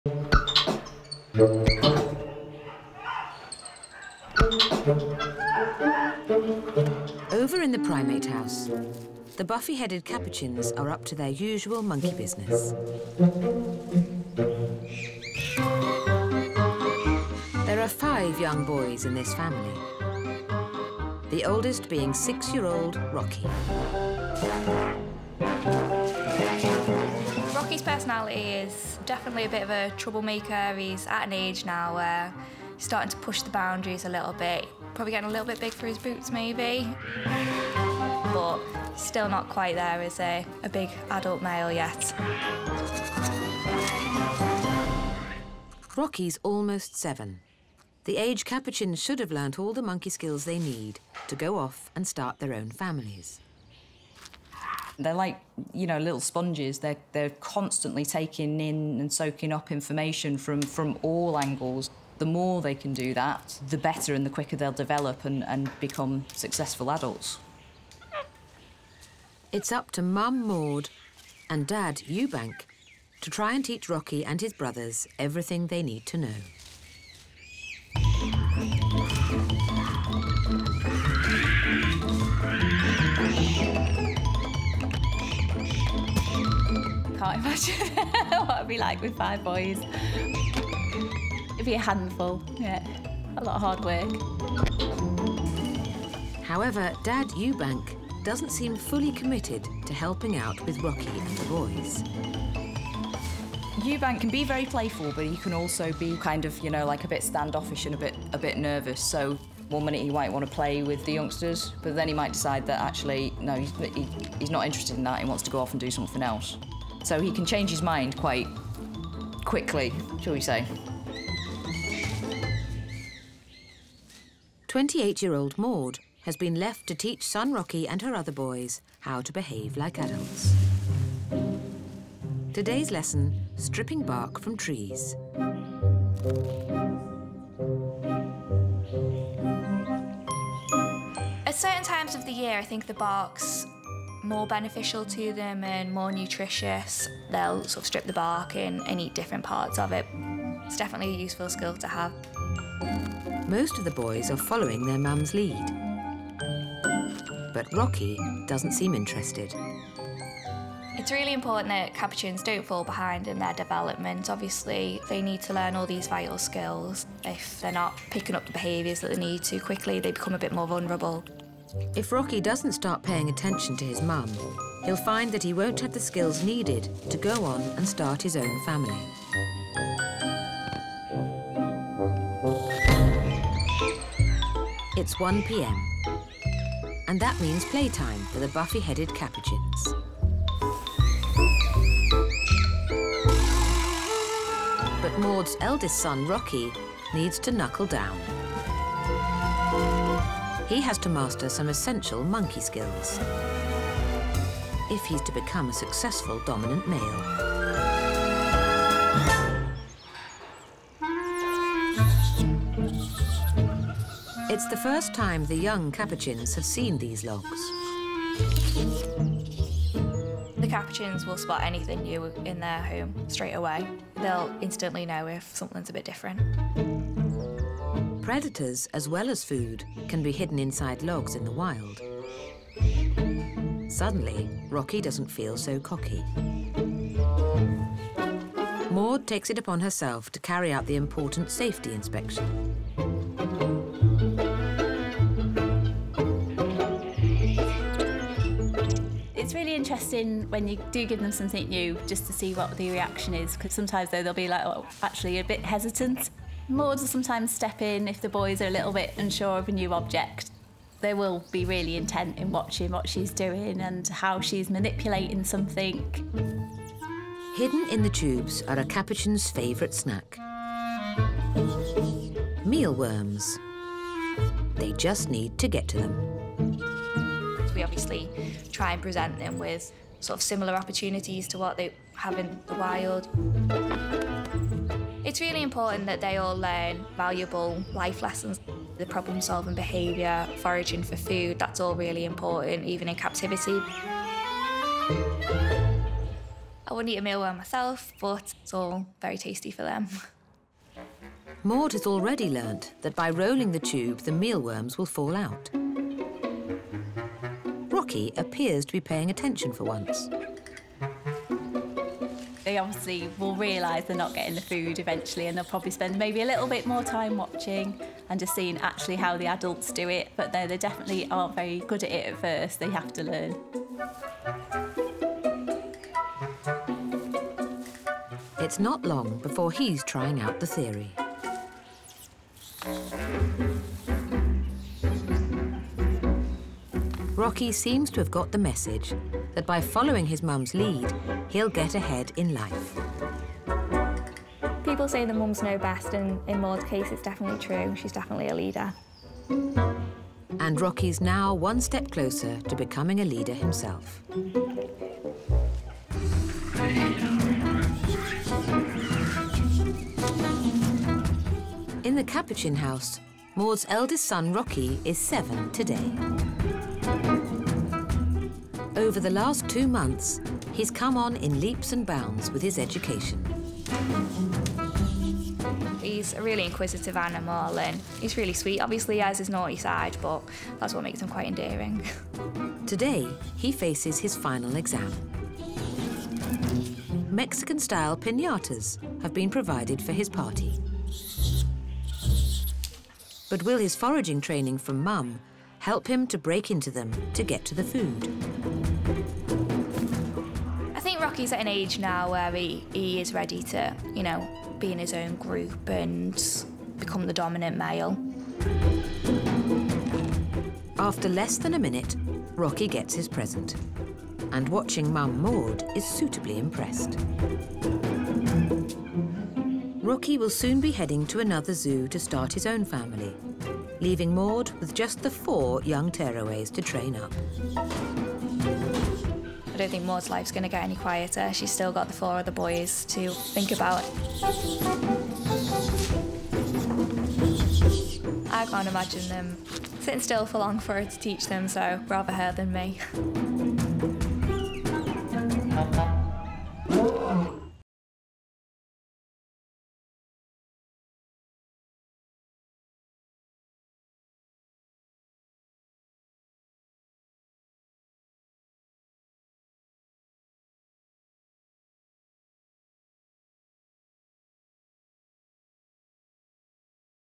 Capuchin Monkeys Playing with Pinatas! | Unusual Animal Behaviour | Nature Bites
Watch the fascinating moment capuchins go through training to become effective foragers - and are gifted pinatas to play with by zoo staff!

The capuchin monkeys are New World monkeys of the subfamily Cebinae. They are readily identified as the "organ grinder" monkey, and have been used in many movies and television shows. The range of capuchin monkeys includes some tropical forests in Central America and South America as far south as northern Argentina